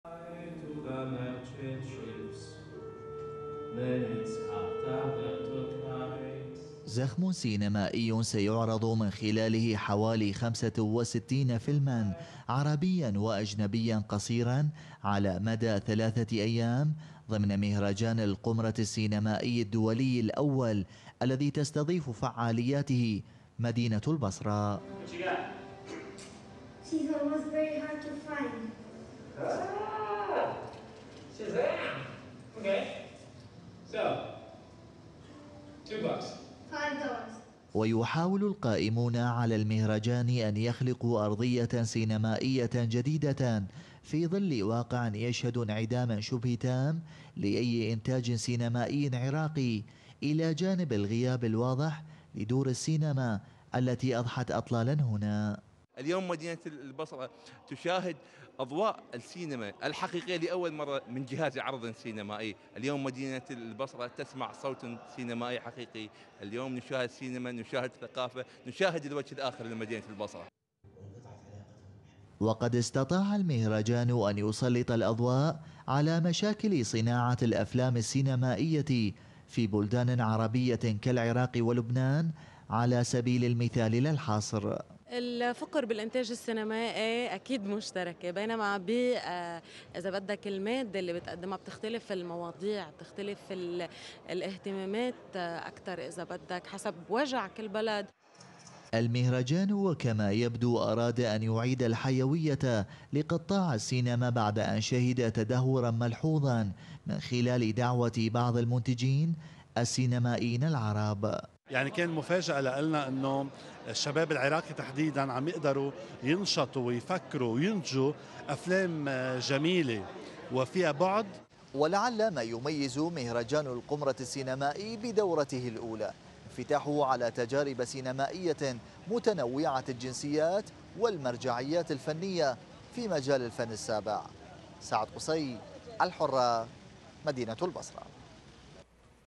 انطلاق اعمال مهرجان القمرة السينمائي الدولي الأول بالبصرة
انطلقت في البصرة اعمال مهرجان القمرة السينمائي الدولي الأول بمشاركة افلام عربية واجنبية حيث سيتم عرض اكثر من 60 فيلما قصيرا على مدى ايام المهرجان الثلاثة   في احدى قاعات الفنادق في المحافظة وذلك لعدم وجود دور عرض سينمائية في البصرة.